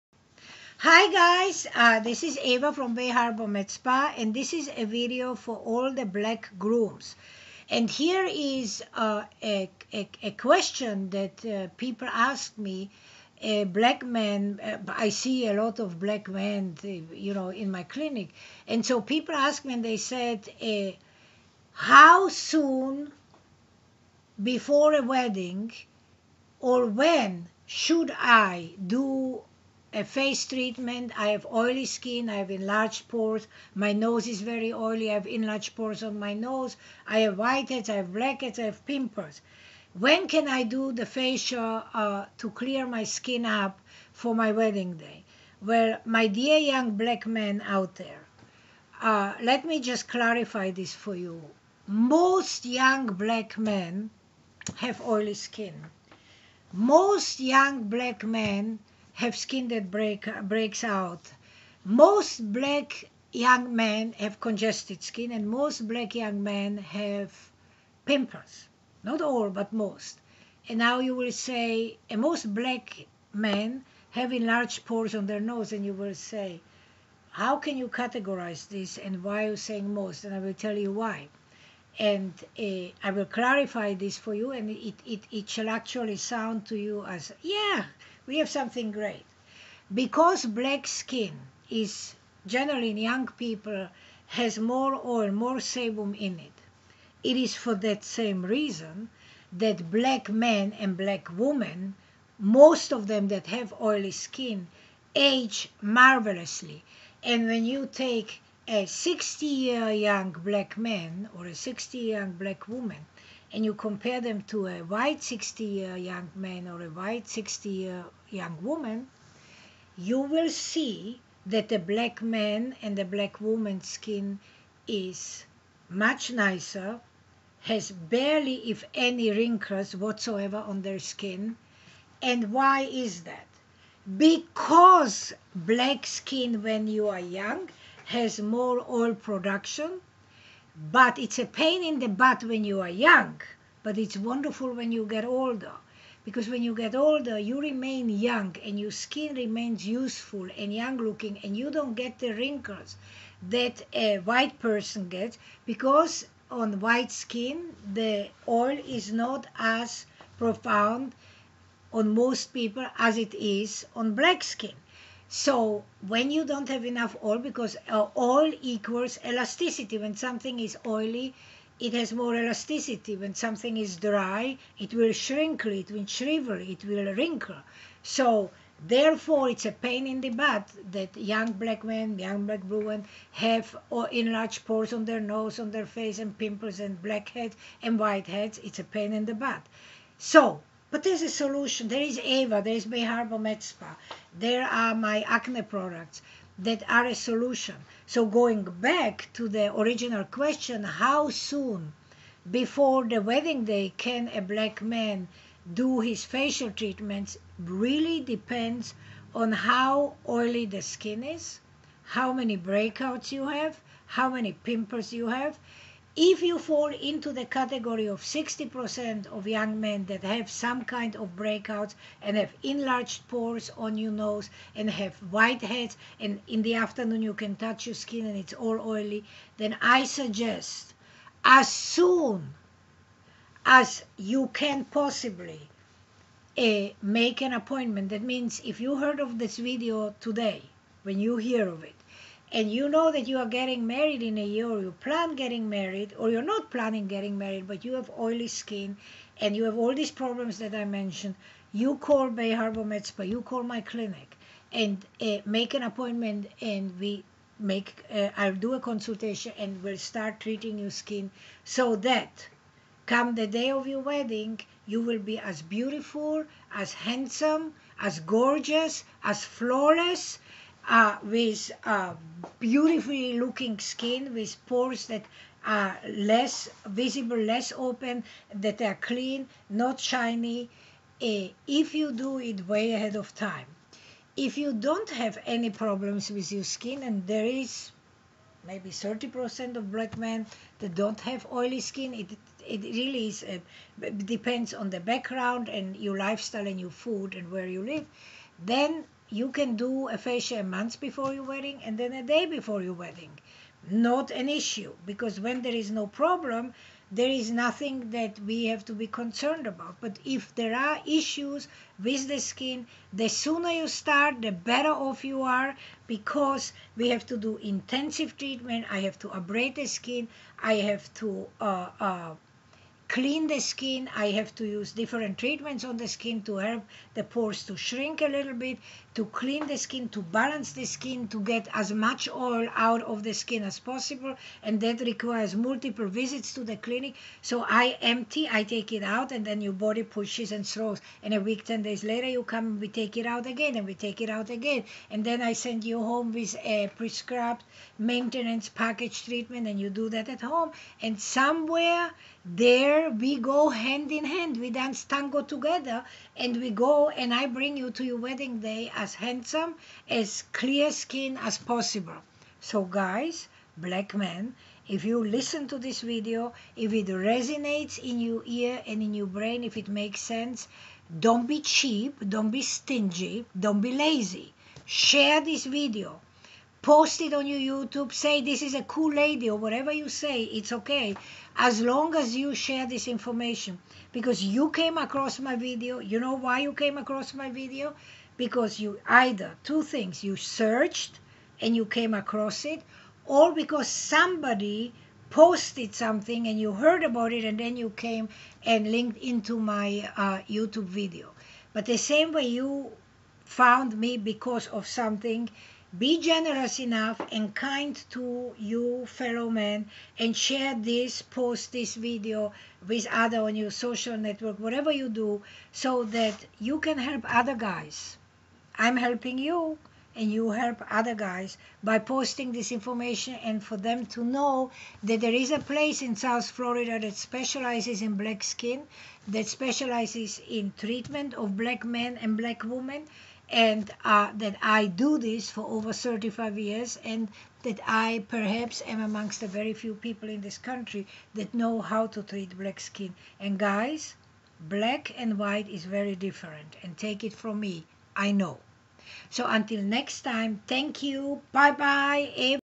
Skincare Advice for Grooms [Black Men] Miami, FL
Today's help with skincare segment is courtesy of Bay Harbour Med Spa (located in Bay Harbor - between Miami Beach and Aventura). The question I am answering today is how soon before your wedding (yes- grooms need to take care of their skin too!) should you get a facial to clear your enlarged pores and oily skin. I hope you find this video to be helpful.